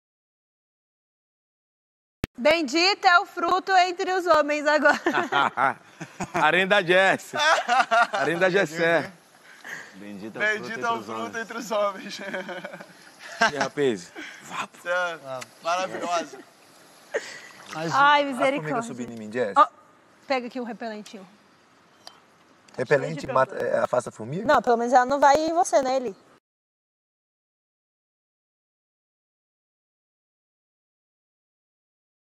BENDITO FRUTO ENTRE OS HOMENS, APÓS CHORAR MUITO ELA BRINCA E RECEBE PICADAS DE FORMIGAS
Após Jesselane chorar muito,  por causa da eliminação de Natália, ela brinca com os restantes dos participantes e diz Bendito o fruto e em seguida fala com Eli que está com picadas de formigas.